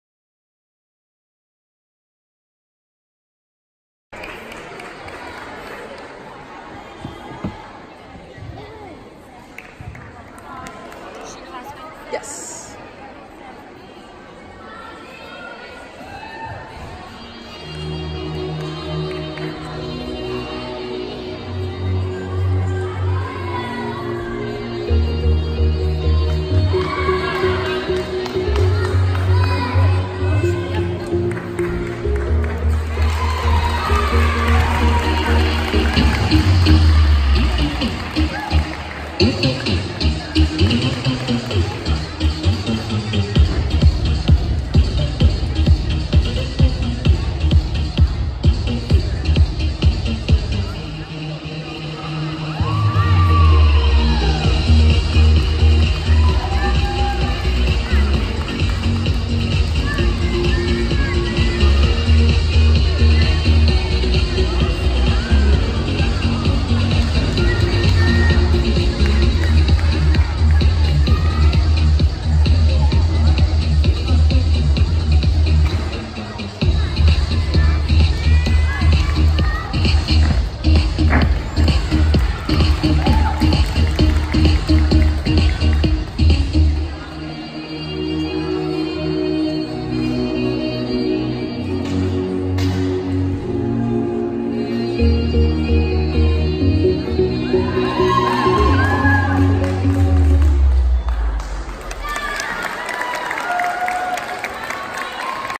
Kaitlyn's Floor Routine 2019 - (Mission Viejo)
We are so proud of Kaitlyn she did great this competition season!

-All Olympia

Video created by: Sophia Wood